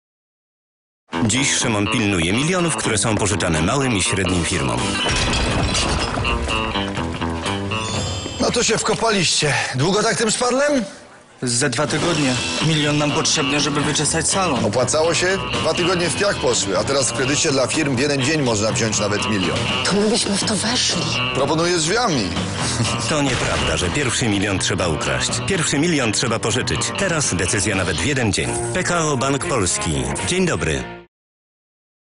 Szymon Majewski ponownie w reklamie PKO BP [marketing-news.pl]
Celebryta Szymon Majewski tym razem w spocie PKO BP wcielił się w postać strażnika bankowego, który informuje niedoszłych włamywaczy, że pierwszy milion łatwiej jest pożyczyć niż ukraść. Reklama promuje ofertę pożyczek dla firm.